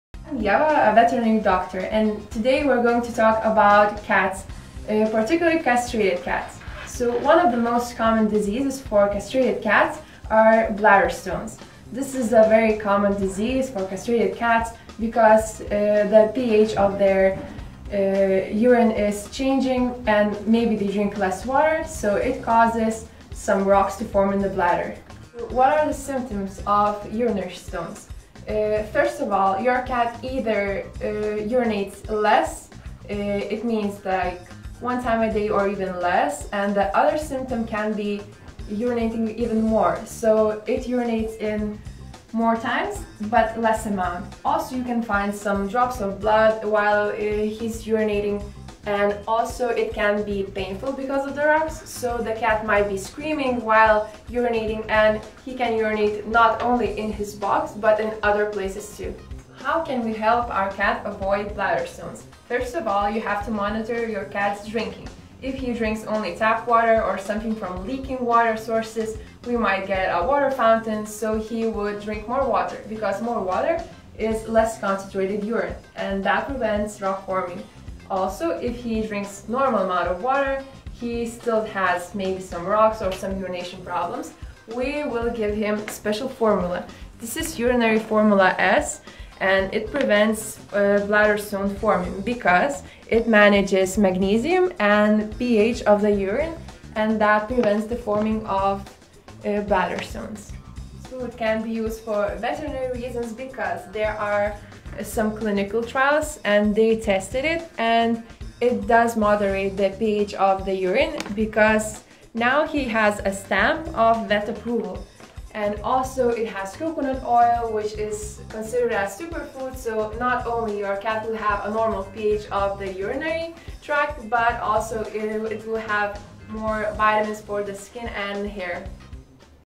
Common problem for sterilised cats.
The main problem that faces more than 70% of sterilized cats is urolithiasis.
We share our exparience how to to recognize urolithasis indications and maintain heathy and happy beloved cat.